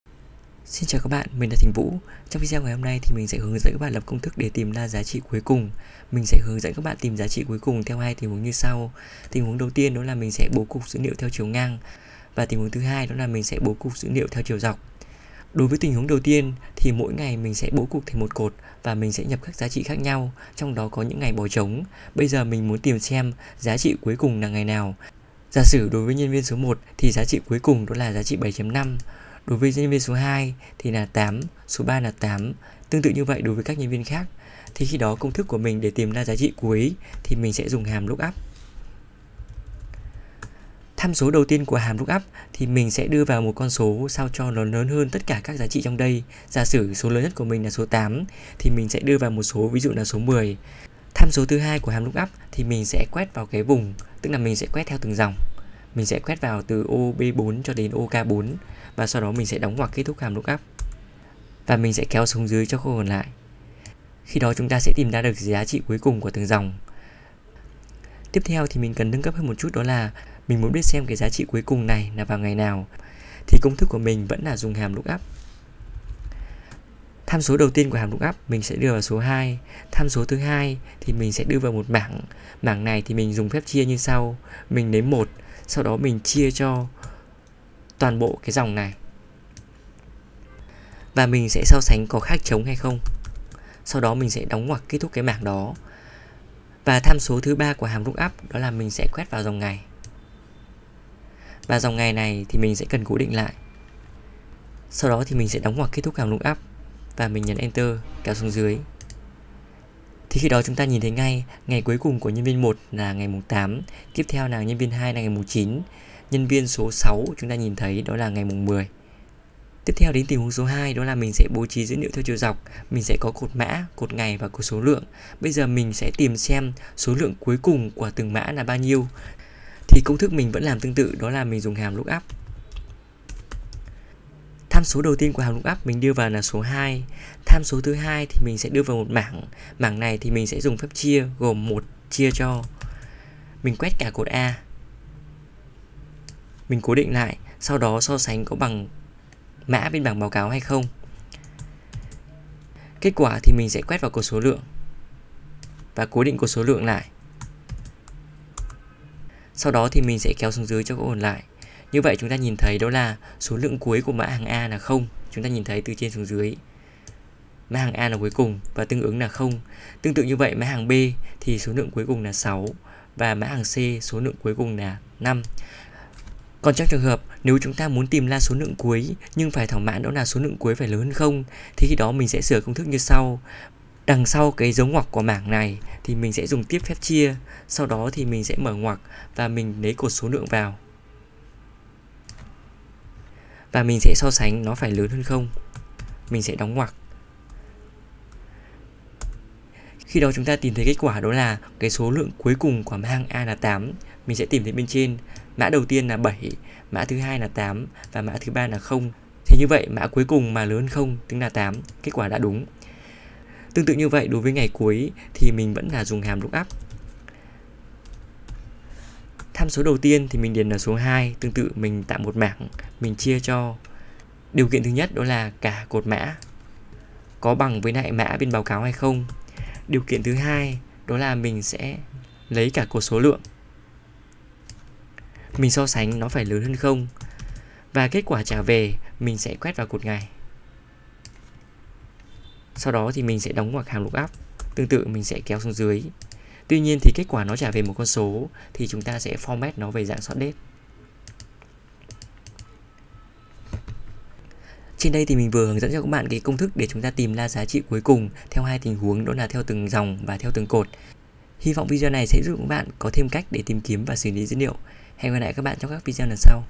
Công thức Excel tìm giá trị cuối cùng theo dòng và theo cột
{Đt+Zalo} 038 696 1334 - Tư vấn khóa học Excel cho người đi làm và doanh nghiệp
Nội dung videos: Sử dụng hàm LOOKUP để tìm kiếm giá trị cuối cùng
- Tìm kiêm giá trị cuối cùng theo từng dòng
- Tìm kiếm giá trị cuối cùng theo cột theo từng đối tượng và thỏa mãn nhiều điều kiện